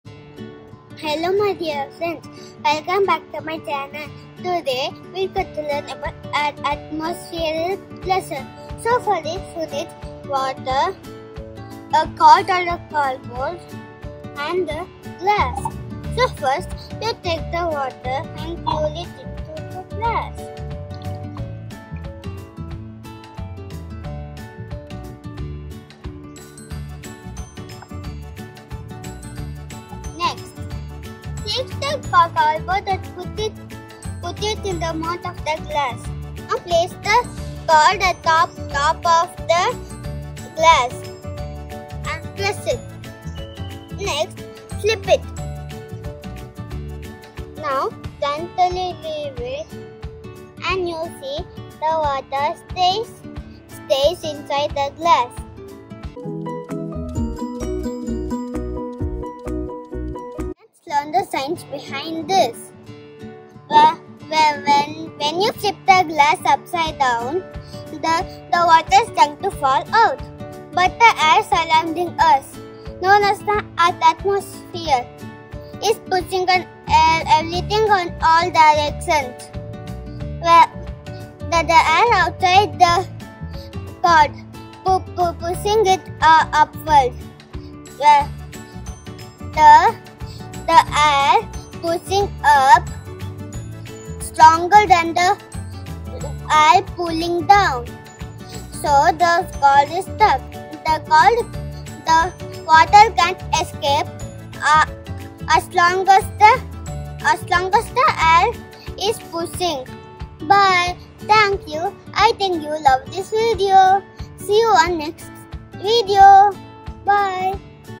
Atmospheric Pressure | Science Experiment | Mithun Baskaran
Atmospheric pressure experiments with water include the upside-down water glass and the rising water in a jar.

To perform the upside-down glass trick, you fill a glass with water, cover it with a piece of cardboard, and invert it, where atmospheric pressure pushes up on the card, holding the water in place.